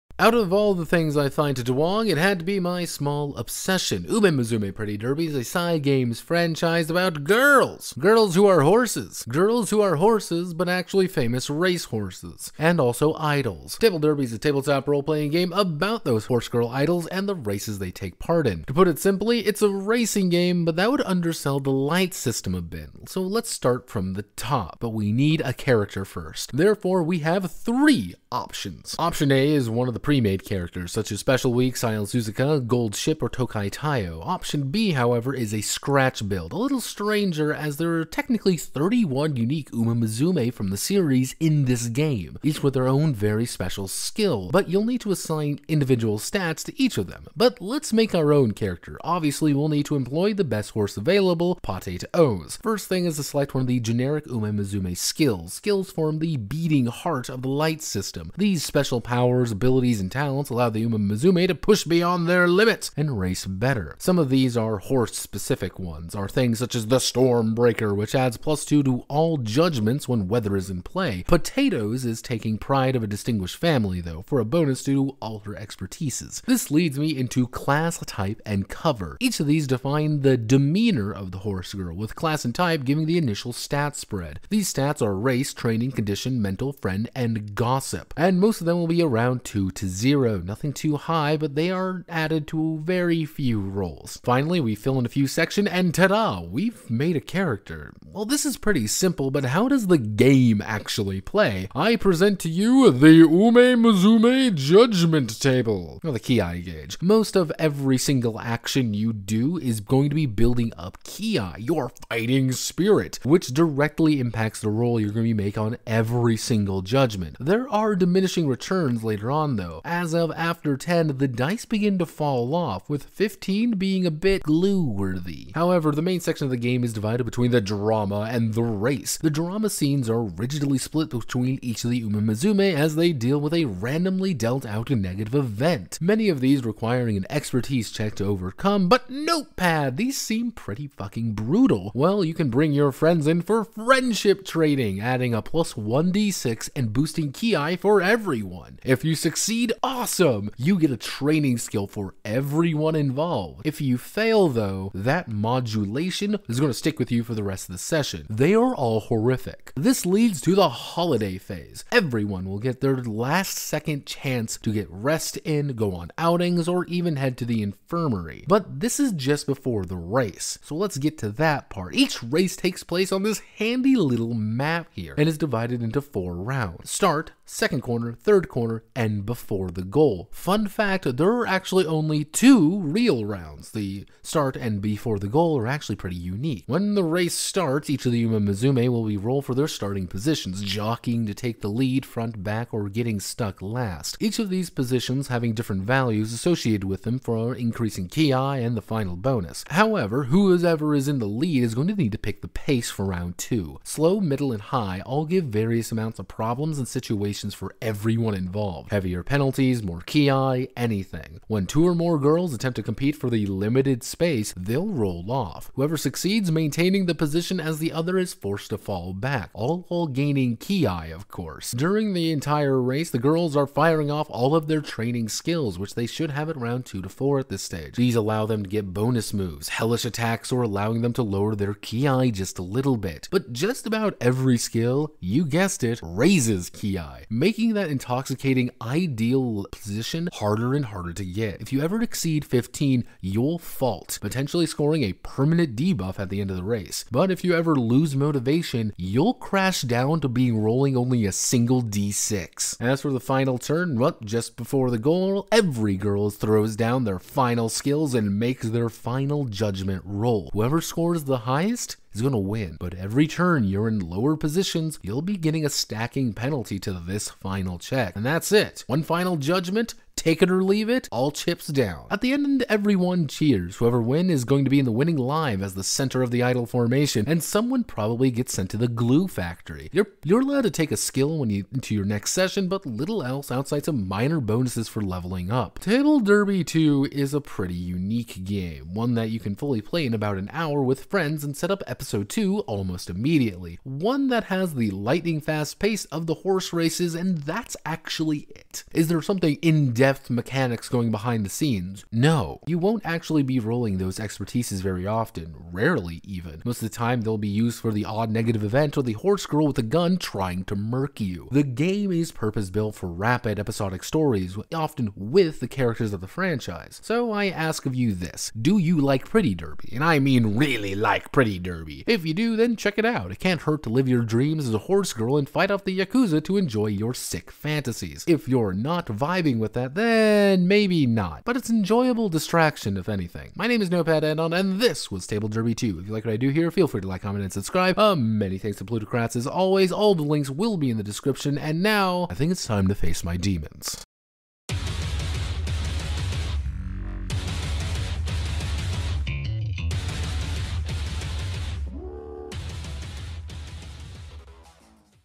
Notepad's DUWANG Detox of Table Derby 2 in About 6 Minutes
My Opinion-Detox of Table Derby 2, a DUWANG Project.

In Short: It's an Horse Racing TTRPG about Horse Girls who are Idols. It does very little else, but does this game need to do anything else?

This is entirely my own opinion and nothing else, the stream can be found in the Playlist.